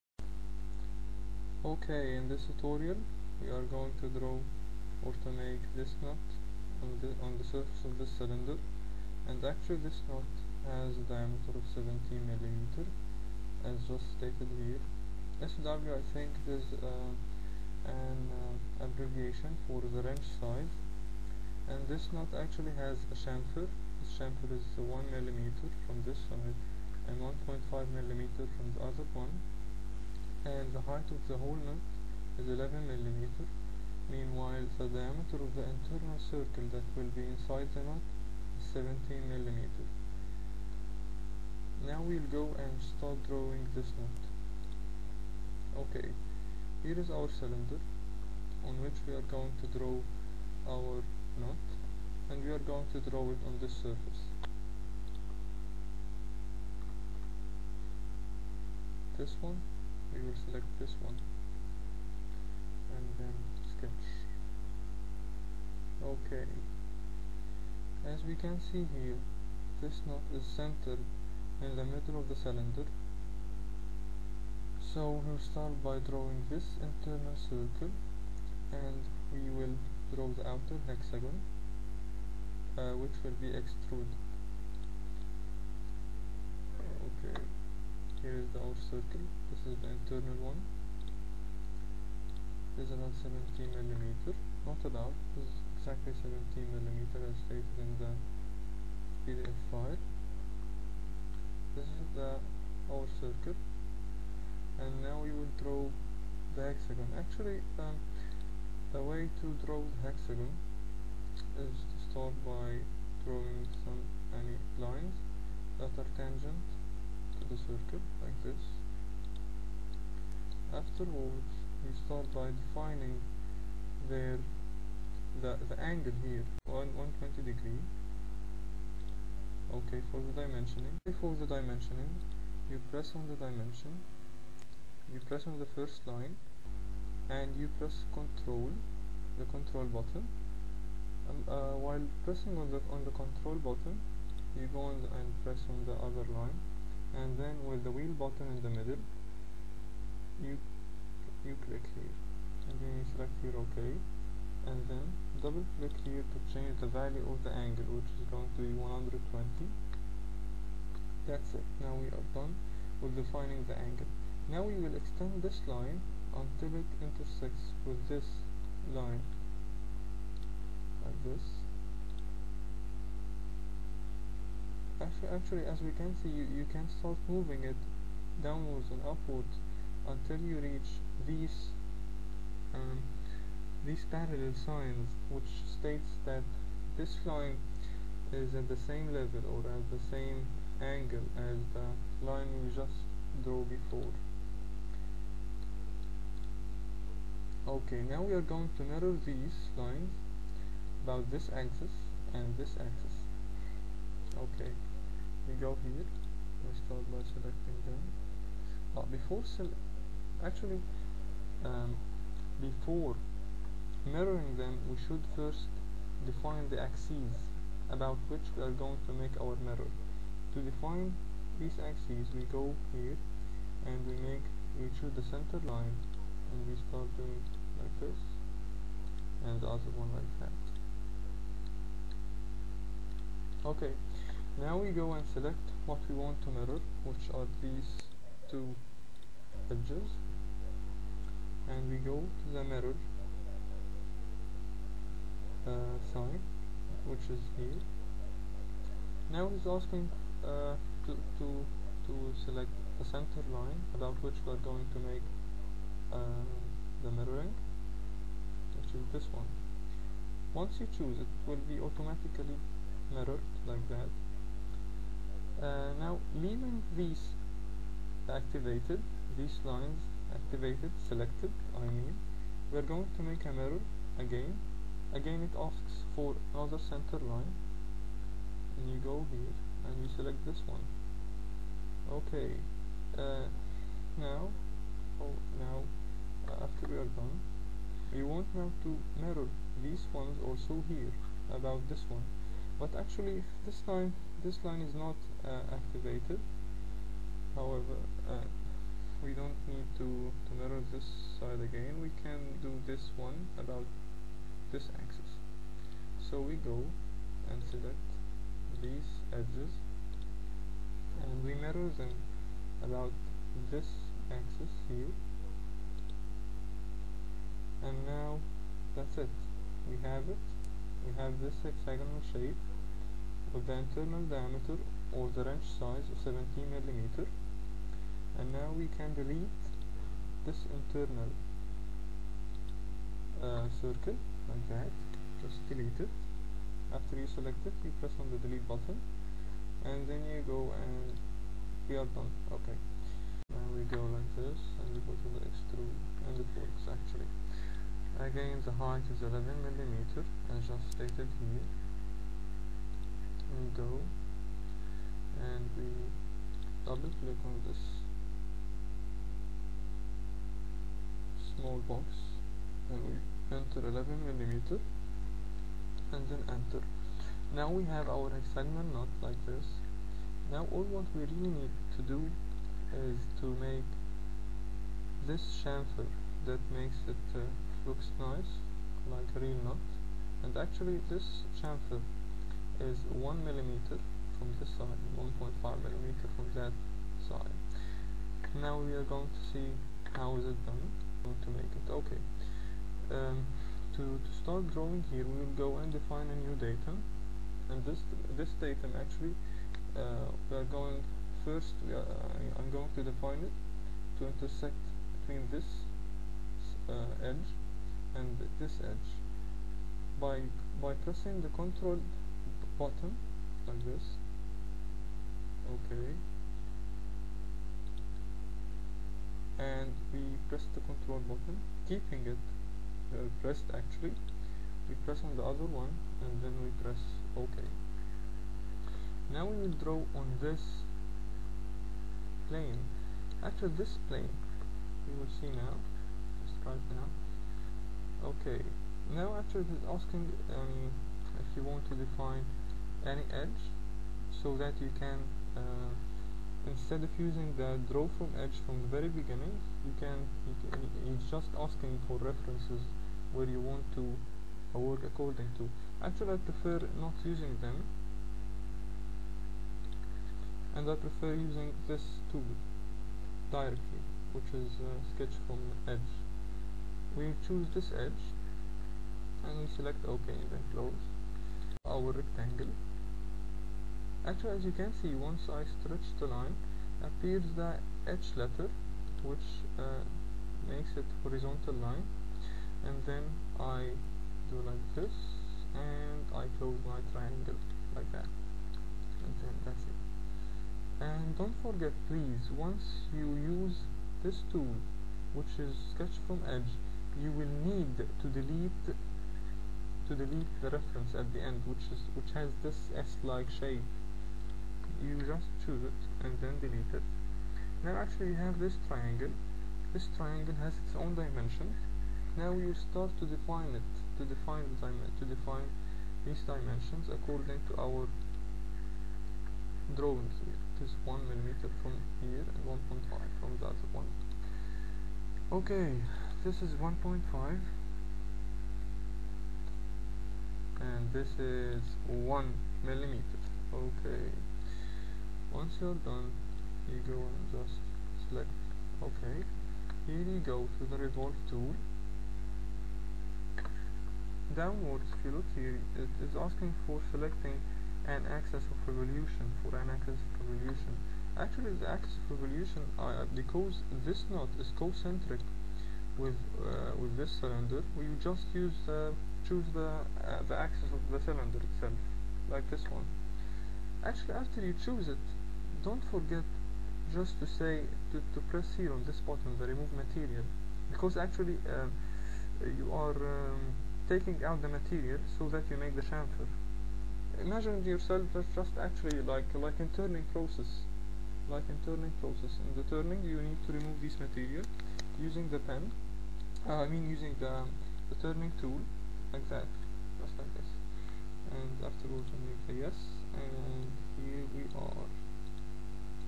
modeling a nut using ProE
In this tutorial, a nut is going to be modeled using a real-look shamfer.